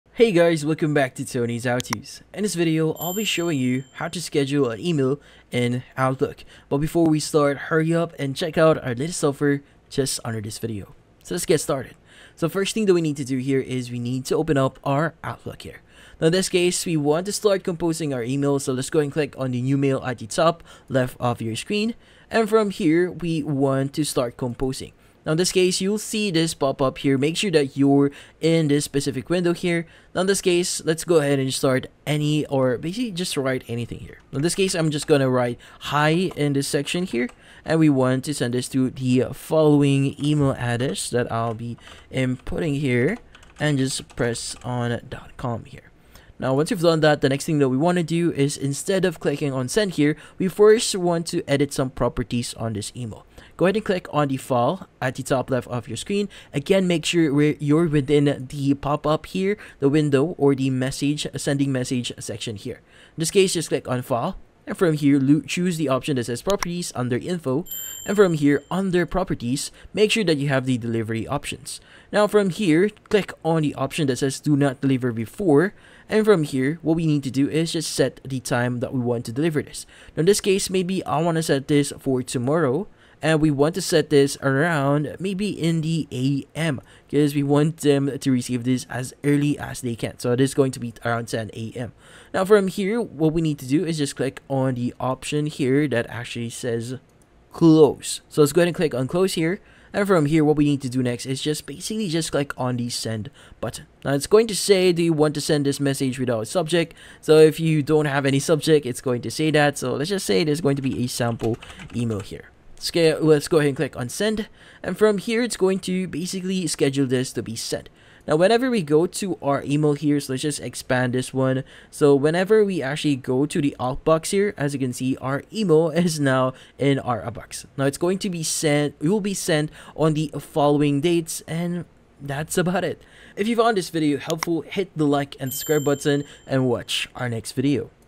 How to Schedule an Email in Outlook (2025)
In this video I show you How to Schedule an Email in Outlook in 2025. The quick and easy tutorial how to schedule recurring emails in outlook. Do you want to know step by step how to send mail later, delay delivery in outlook in 2025? Yes? Great!